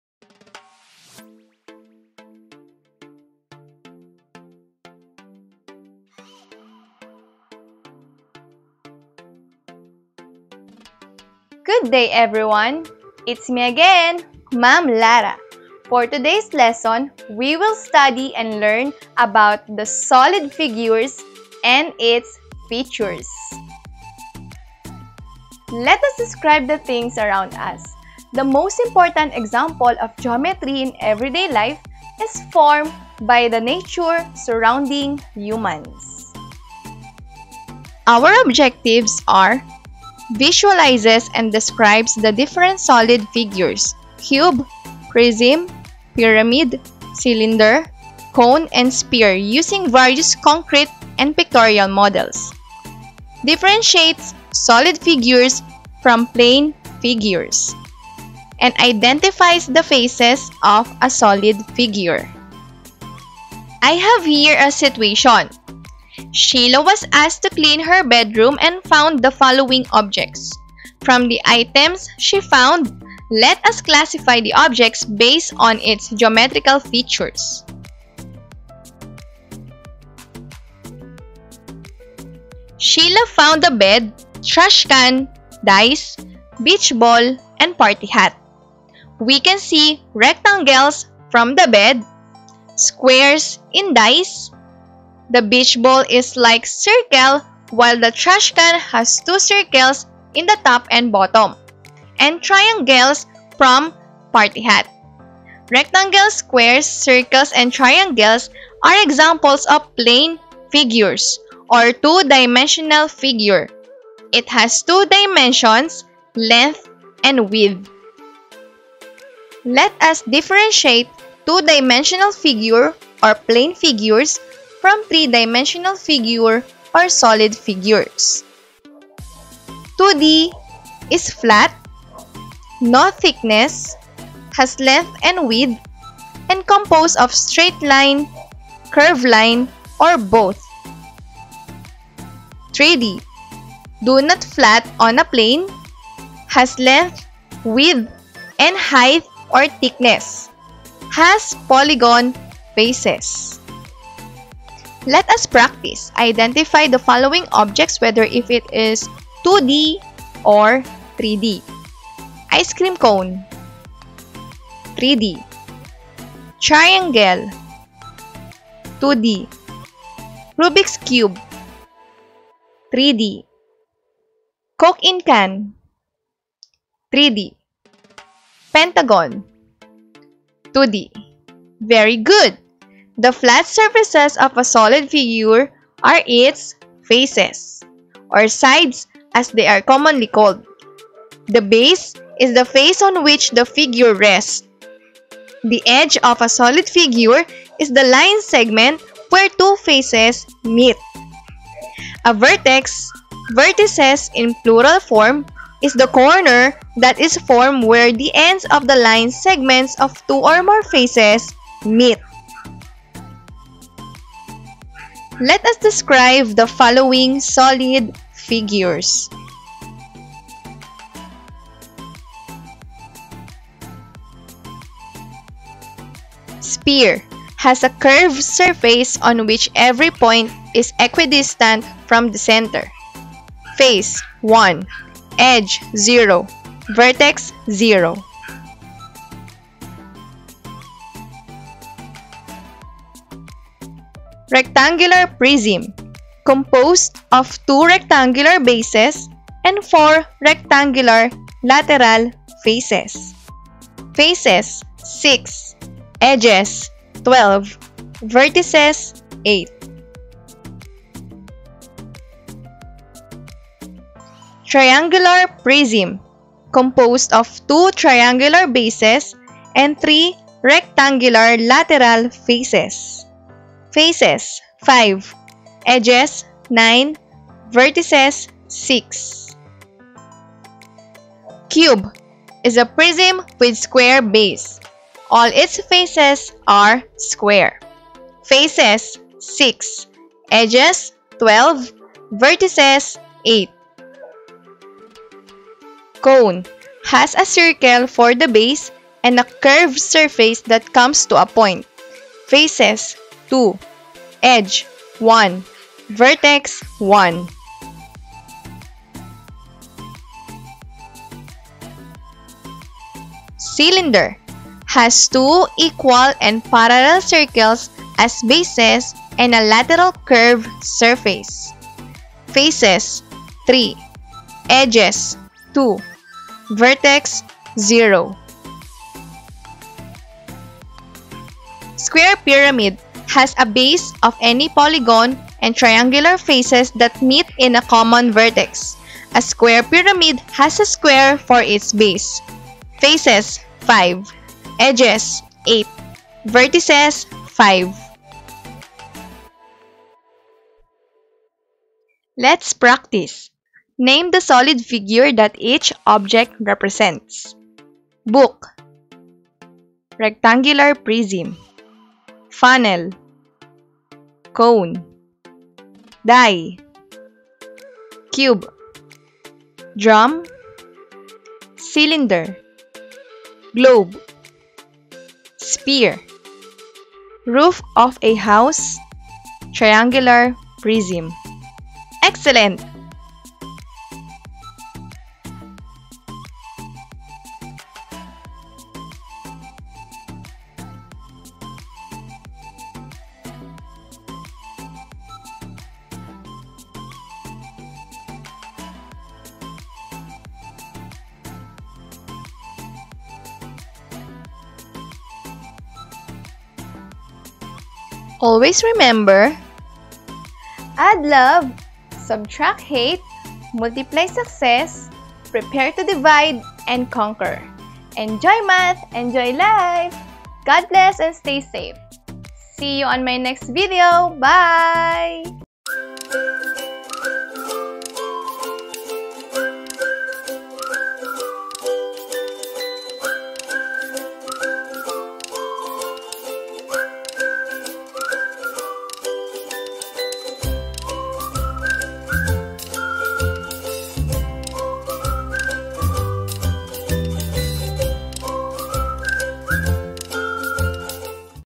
MATH 6 Quarter 3 - Solid Figures and Its Features
Objects in the real world are called three-dimensional if they have length, width, and height. In mathematics, the three-dimensional objects are known as solid figures.